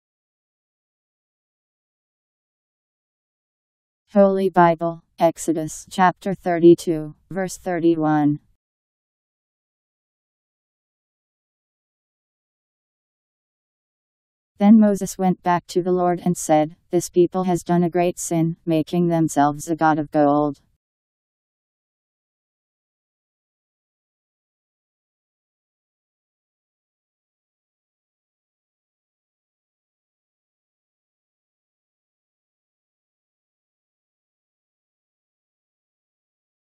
Holy Bible Exodus Chapter 32 Verse 31
Video shows Holy Bible Exodus Chapter 32 Verse 31: Then Moses went back to the Lord and said, This people has done a great sin, making themselves a god of gold; Powered by MaryTTS. Text taken from Bible in Basic Edition